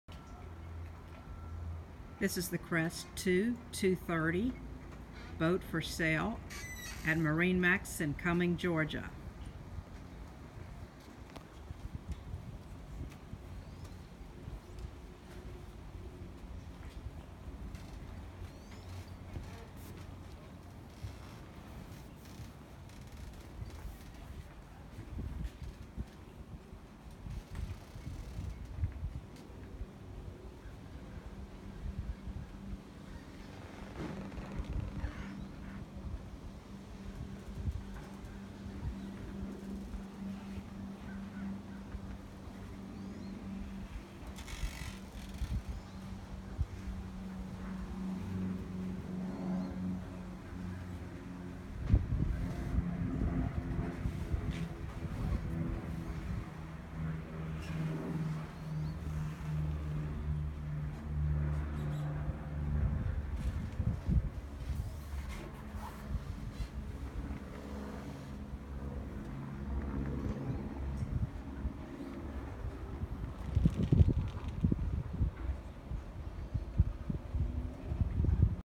2018 Crest II 230 For Sale at MarineMax Cumming
Don't miss this beautiful, elegant and comfortable Crest II.  Laid out in the popular "L" style with sunpad and changing room.  You'll enjoy keeping your captains drink ice cold while relaxing with friends and family.  The powerful 150 on the back will run you in the mid-30's and easily pull your skier or tube.  Come see this one today at MarineMax in Cumming!